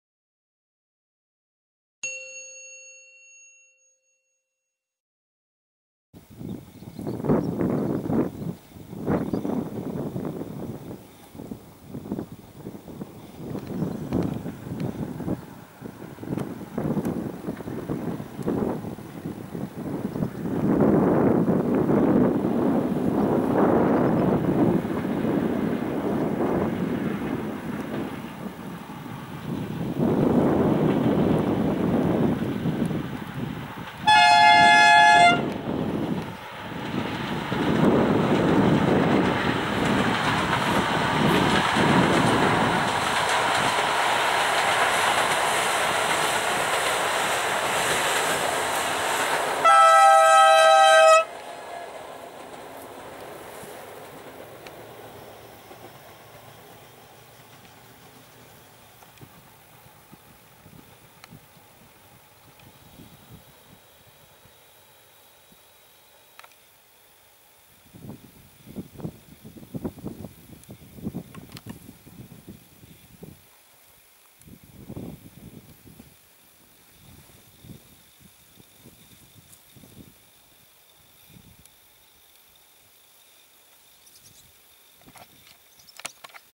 [PR & Lotos] 6Dg-149 z 78101 "RYBAK" || Dąbrówka Wielkopolska
6Dg-149 z pociągiem "RYBAK" 78101 Poznań Główny - Wierzbno.
Warto dodac ze skład odmiennie prowadzony był lokomotywą 6Dg-149 należąca do Lotos Kolej.